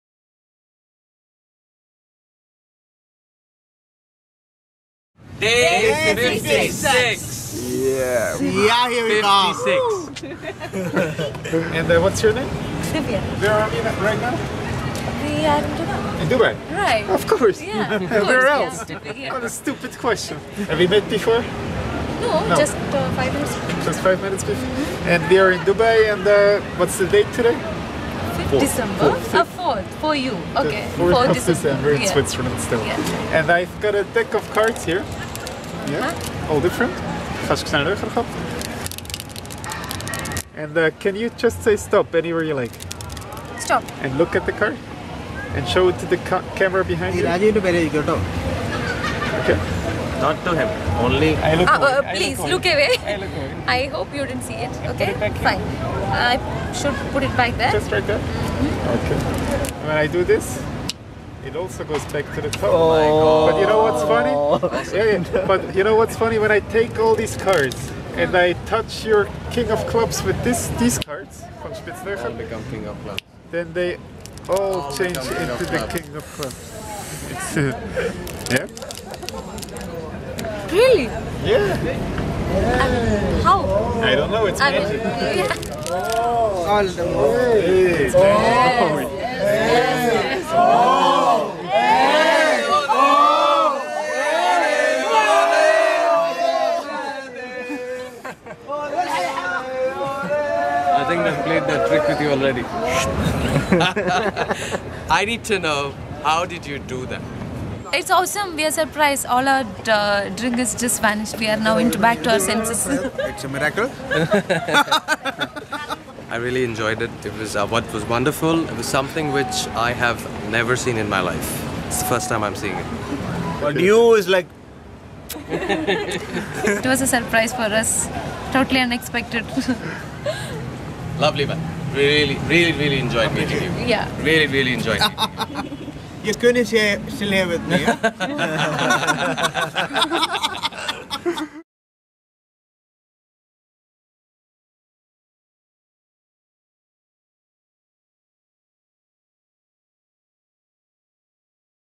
Day 56 - Dubai, Ein Kartenspiel verwandelt sich in lauter gleiche Karten
Zauberkünstler Florian Klein präsentiert täglich ein Zauberkunststück

Kameras werden von Zuschauern geführt.

Schnitt und Upload: Florian Klein

Equipment:
2 x GoPro Hero 2, Zoom Q3HD, 2 x Canon Powershot S100

Schnitt in Avid Media Composer